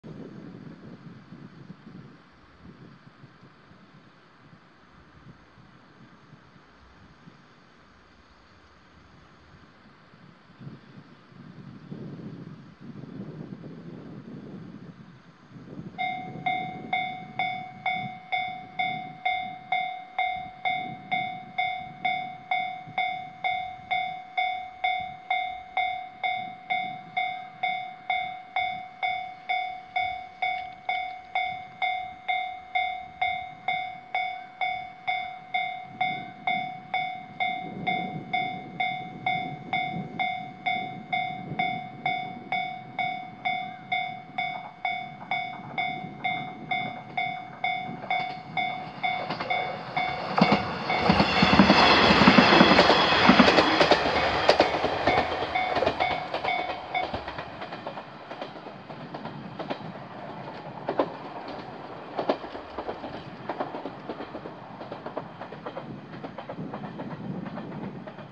【踏切】JR飯山線 C型の田んぼ踏切 (Railroad crossing in Japan)
信濃平～戸狩野沢温泉駅の間にあります。
大塚柳新田踏切です。